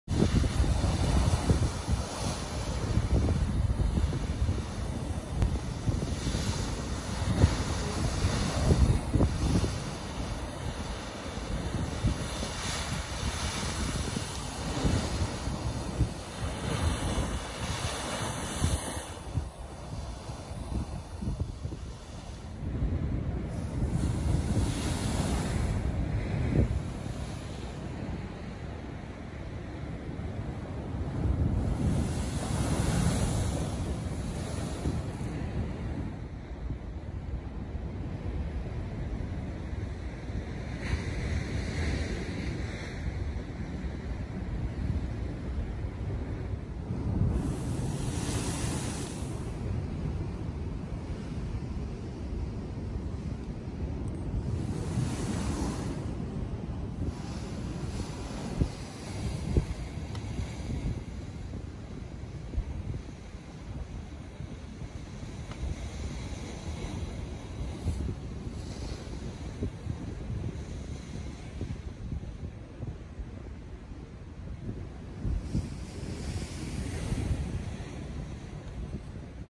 Lake Michigan’s icy waves
Watching rolling waves of ice at Whiting’s Lakefront Park was fascinating. Getting out of the car for a closer look was even better.